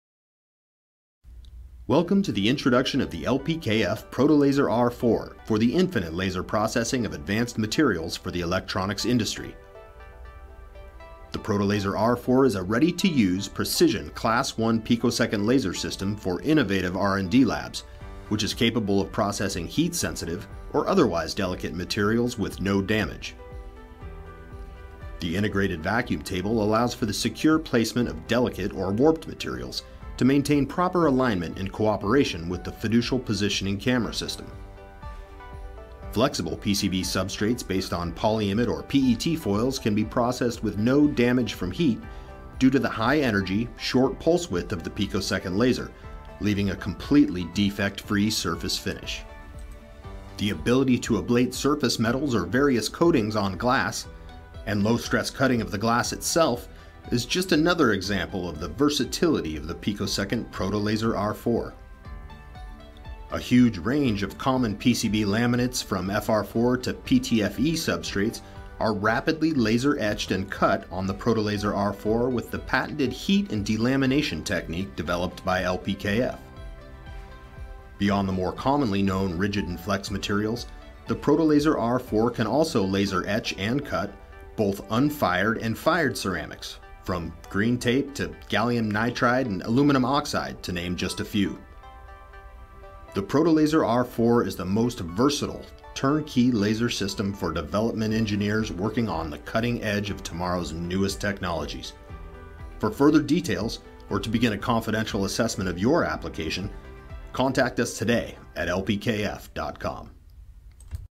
LPKF ProtoLaser R4
The LPKF ProtoLaser R4 is a turn-key picosecond laser for micro material processing. This 515nm (green) laser is capable of performing delicate ablation of advanced substrate materials such as fired or unfired ceramics, thin films and vapor deposited coatings to name just a few. for the full data sheet and additional information.